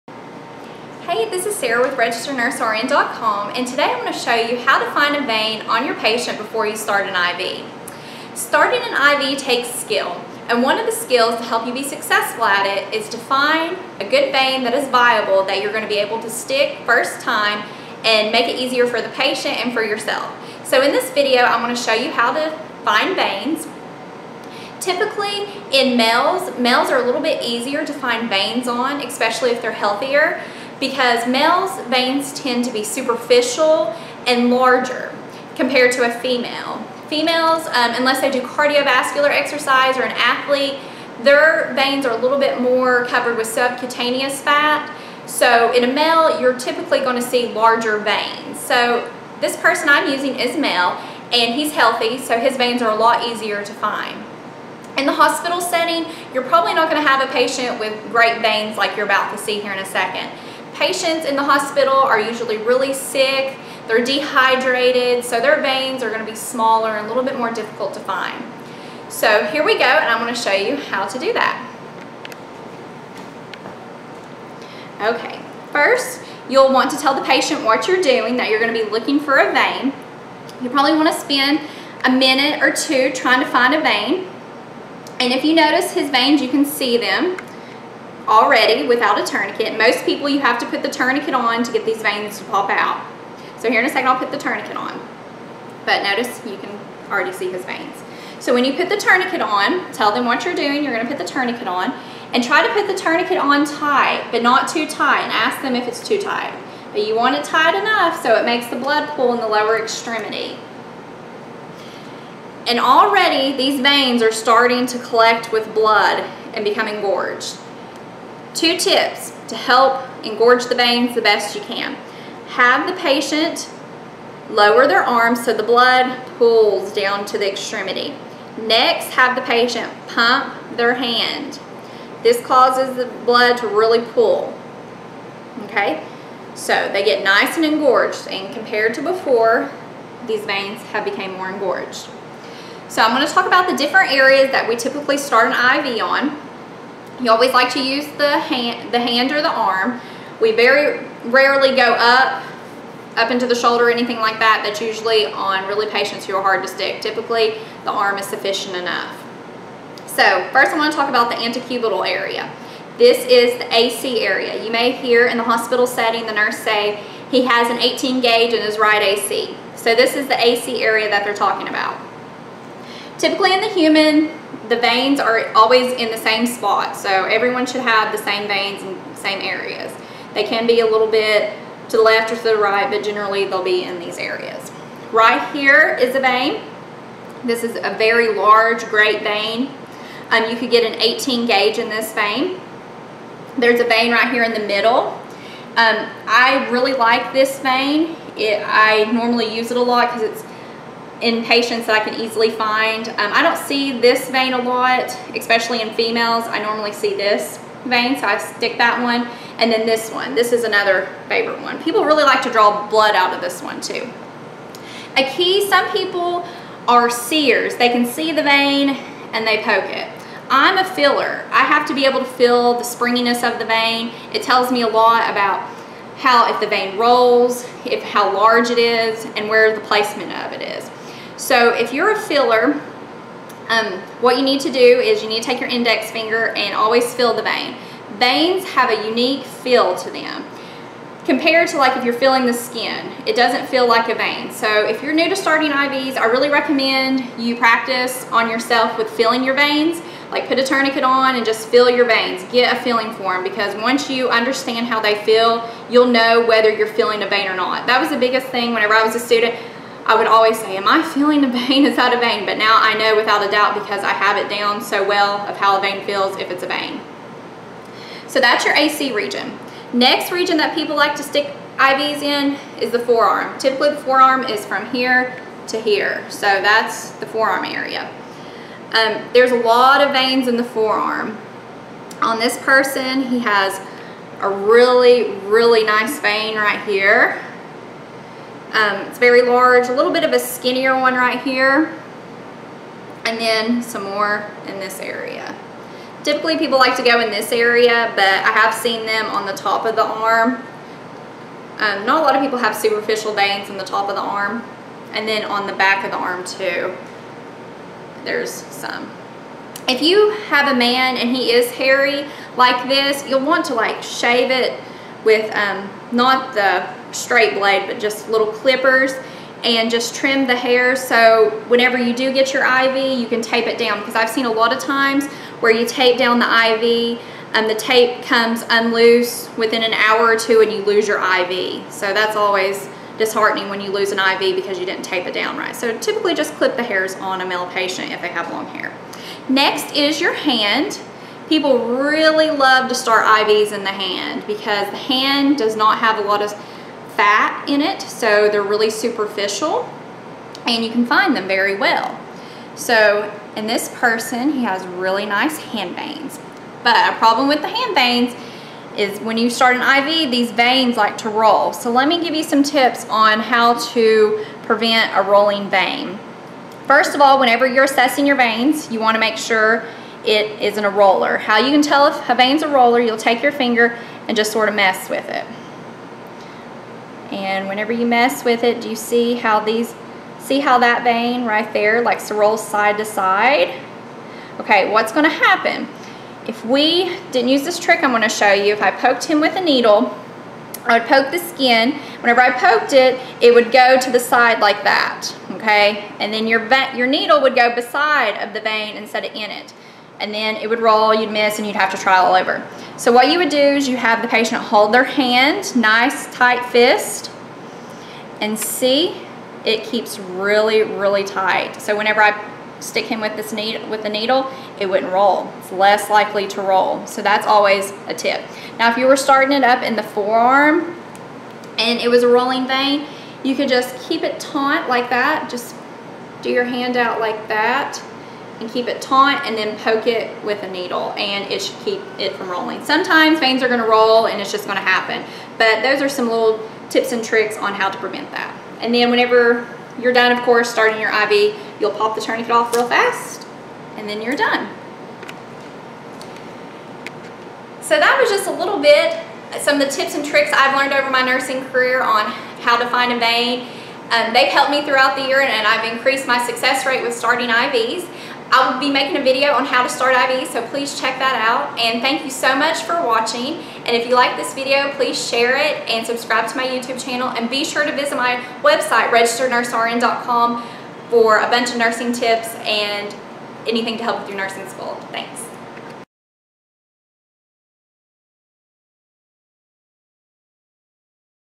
How to Find a Vein When Starting Ivs or Drawing Blood Tips in the Arm
Learn how to find a vein using a tourniquet when drawing blood or starting an IV in the arm (antecubital AC area). This video is a teaching tutorial for nursing students, new nurses, or phlebotomists on how to find the cephalic, basilic, and medical cubical veins in the antecubital area of the arm.

Finding a vein can be difficult when you are a new nurse, phlebotomist, or healthcare professional. Some patients have small veins covered in a layer of subcutaneous fat. In this video, I demonstrate tips to find a vein using a person with very visible veins.